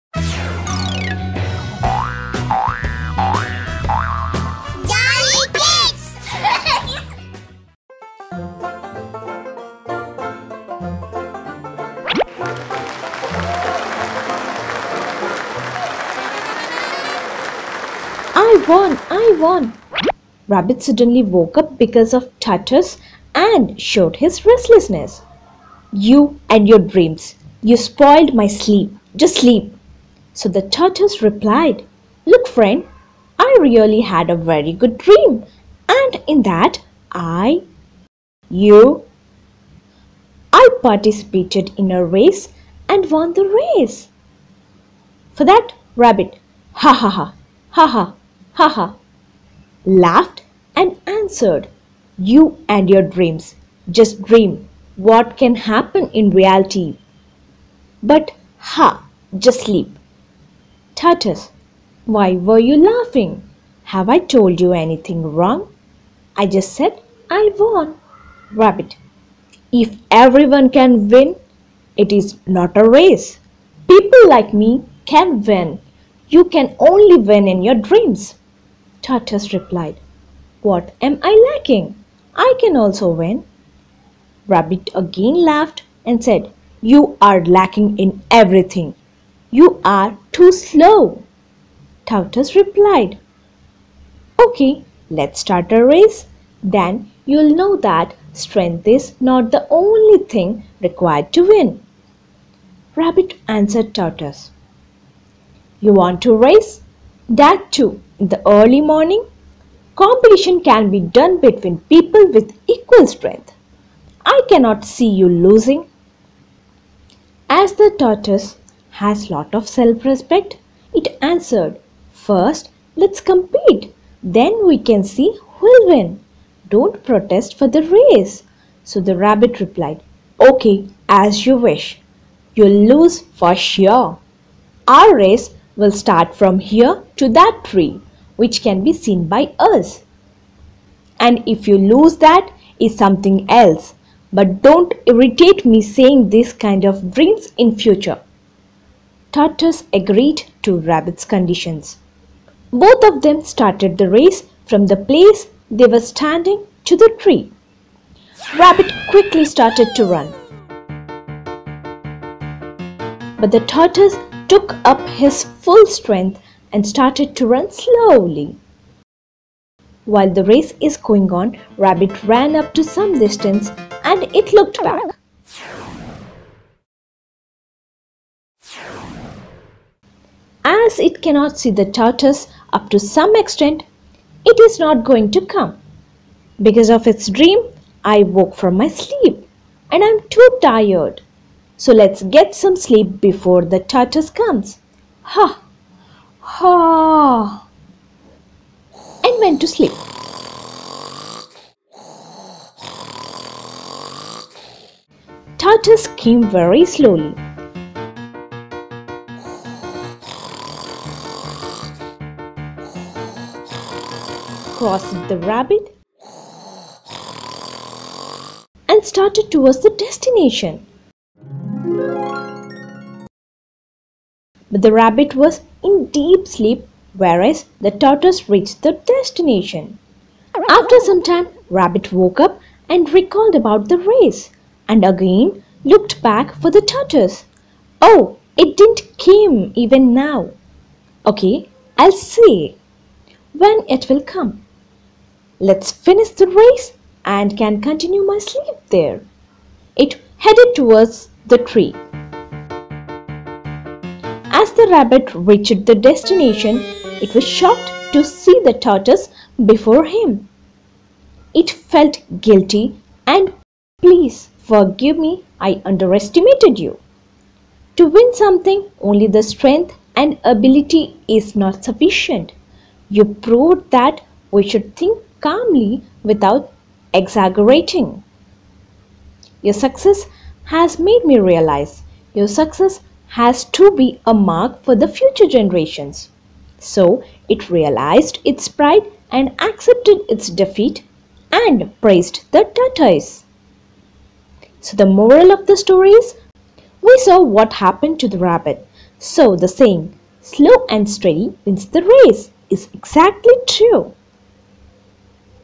Rabbit and Tortoise Story in English | Moral stories for Kids | Bedtime Stories for Children
Hello Kids! Welcome to Jolly Kids English.
Watch Rabbit and Tortoise Story in English | Moral stories for Kids | Bedtime Stories for Children.

Watch our videos in other languages